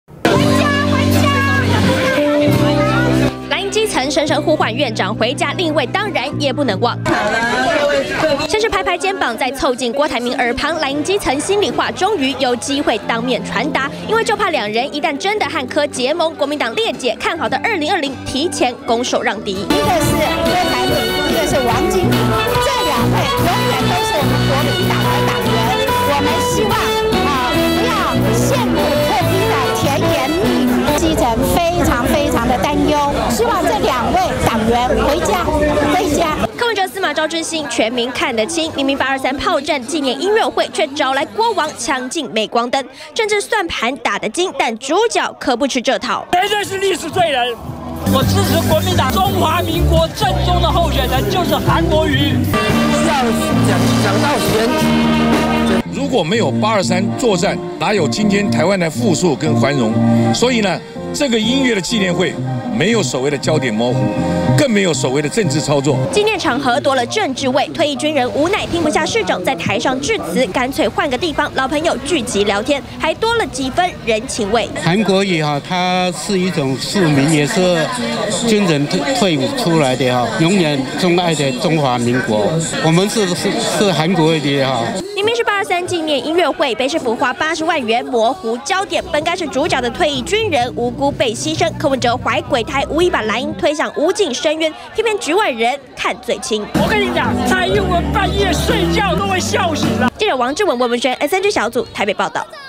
20190823中天新聞 823紀念會竟找郭、王！退役軍人嗆柯「模糊焦點」
郭柯王8/23同框出席，823砲戰紀念音樂會，被外界視為，要藍營裂解 2020埋分裂原子彈，活動上的主角「退役軍人」，不滿認為柯文哲模糊焦點，更喊話郭台銘、王金平，別成「歷史罪人」，藍營基層也把握機會，當面向郭王喊話「回家吧」。